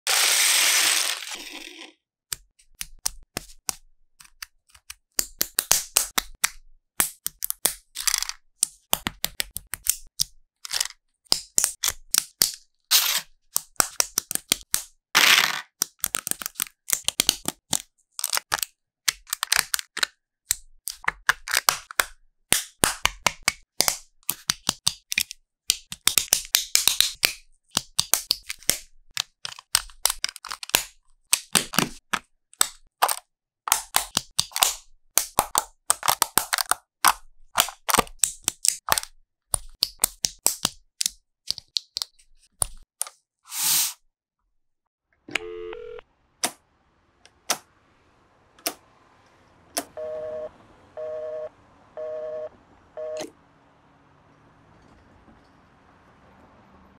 Guess what am I building with lego
It’s a rotary pay phone📞! I have been working on some other projects lately, finally had some time to film this interesting piece. The dial mechanism is actually quite simple, although it took me a whole weeks to figure it out. Go watch the video and you will find out what kind of tricks I was using.